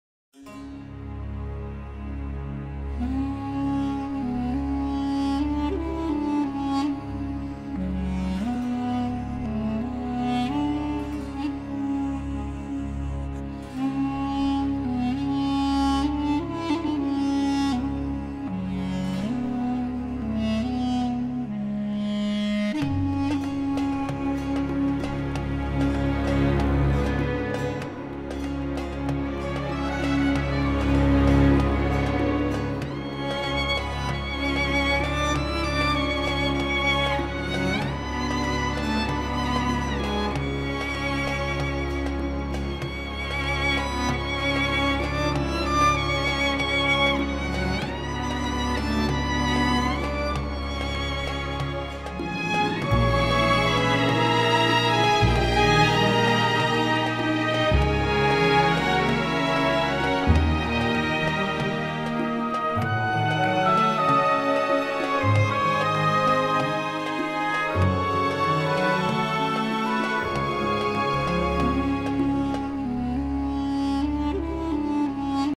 Art, beauty and symbol in the metro station from Istanbul
See what art, beauty and symbol you can see in metro station from Istanbul - Turkey